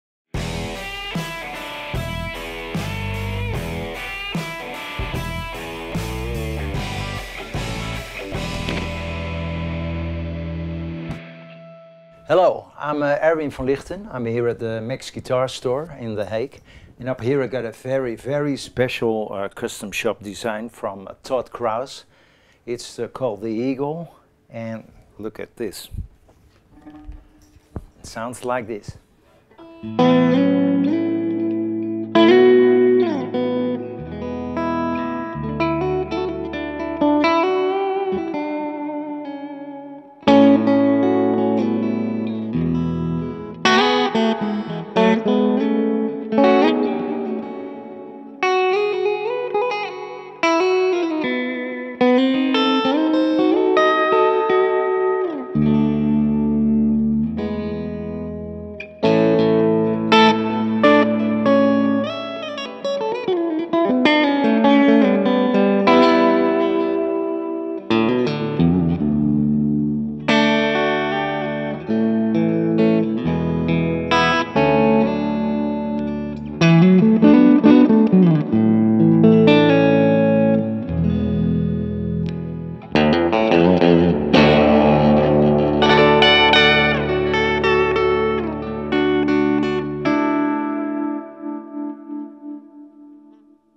Fender CS Masterbuilt Todd Krause 'The Eagle' Stratocaster (SOLD!)
This playable piece art is the brainchild of Fender Custom Shop masterbuilder Todd Krause and renowned artist Darren Vigil Gray. Darrens is a well known artist in the USA and his paintings go for anywhere between 4000 and 10000 euro each!